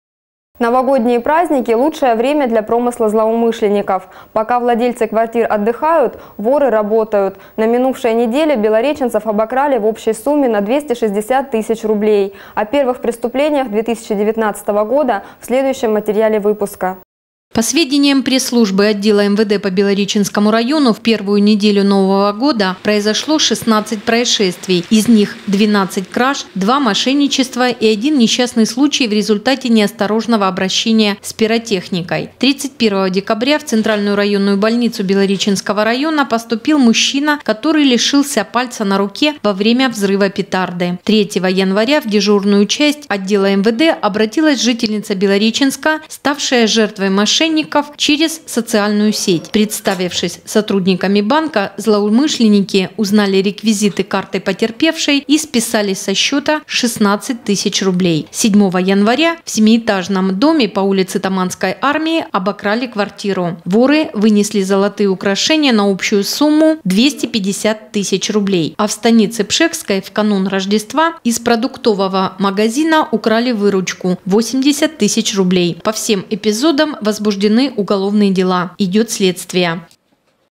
Происшествия новогодних праздников
Праздники  — лучшее время для промысла злоумышленников.